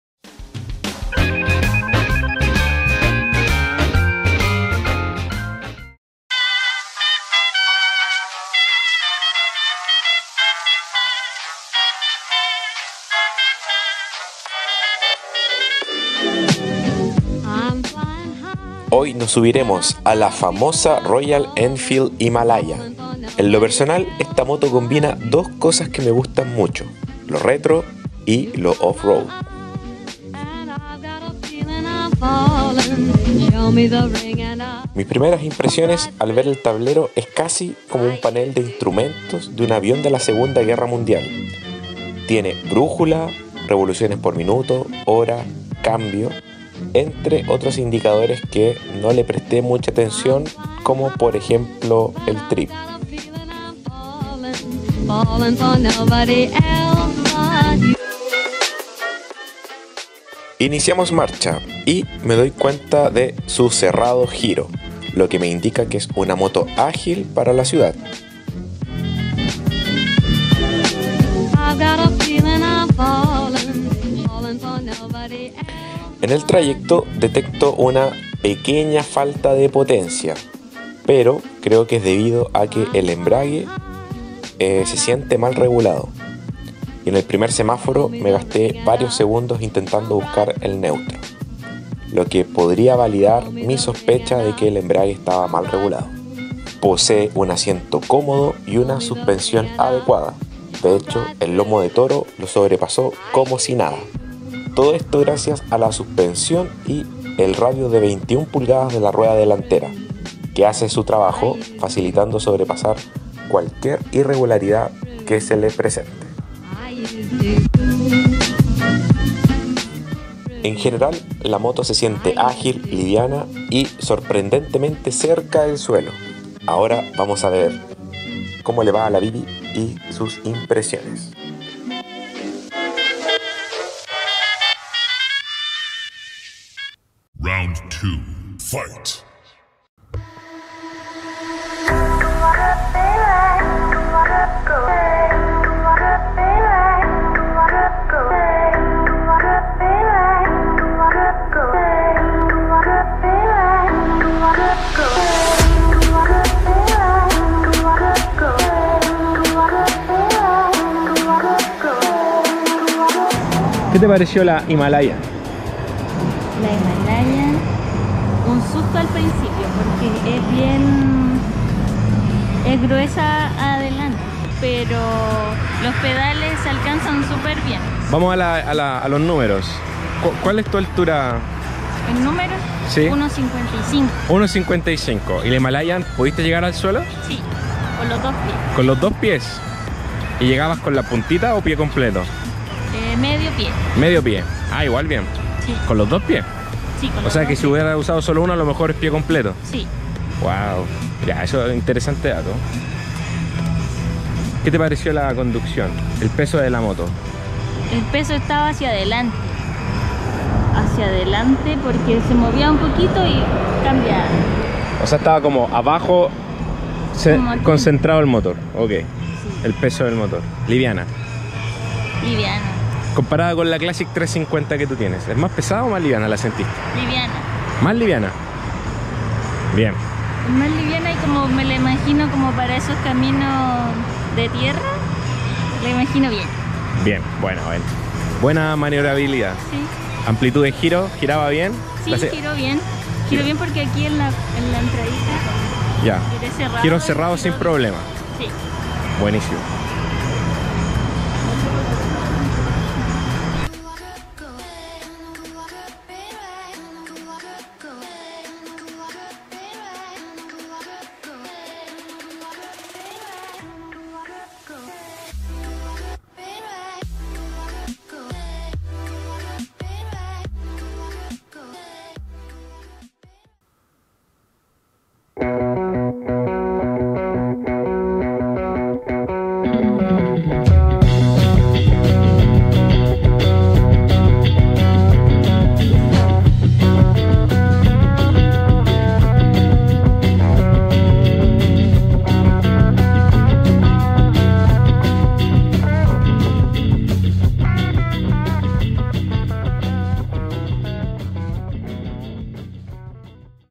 Test Drive Royal Enfield Himalayan b6
hoy participamos de un testdrive de la Royal Enfield Himalayan
validamos que una persona de 1.55 metros llega sin problemas al suelo :D